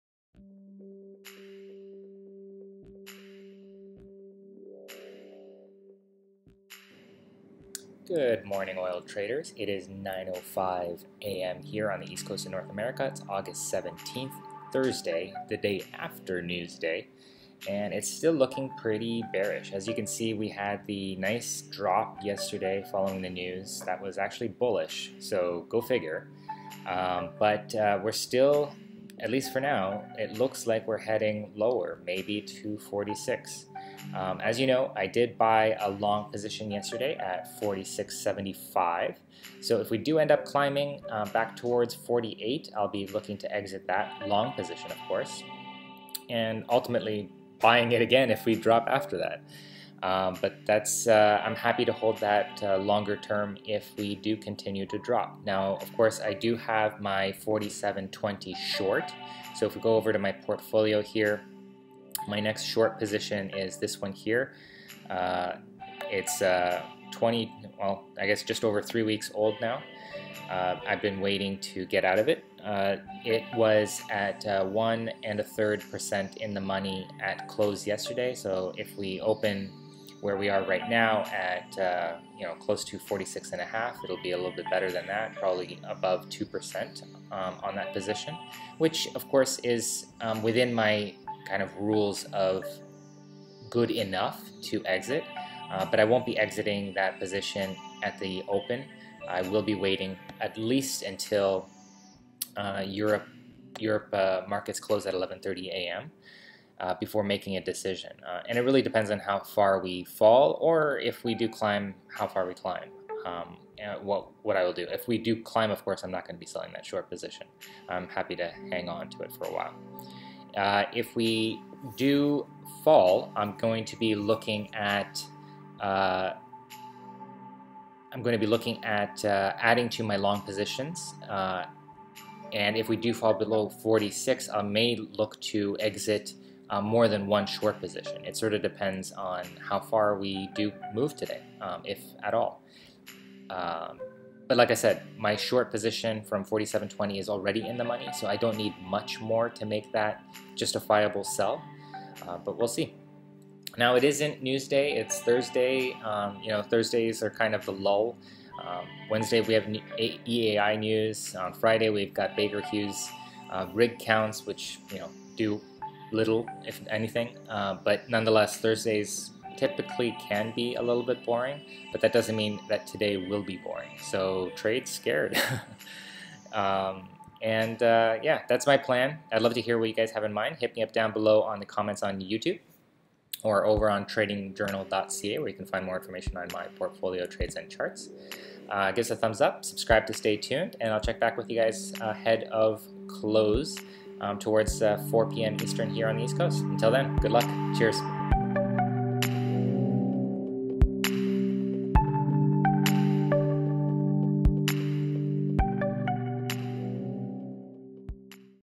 93 - Game Plan for Aug 17, 2017 - Should be able to profit from whichever direction we choose today.
* It still looks bearish this morning, but anything is possible!
* My long buy at $46.75 may end up getting sold if we climb back toward $48
* I have a short at $47.20 that is nicely in the money already, so if we drop farther, then I’ll be exiting that position…
    * May also exit an additional short if we fall below $46
    * If we do fall below $46, I’ll be adding to my longs, again
* It’s not news day, but that doesn’t mean it can’t be exciting!
    * Trade scared!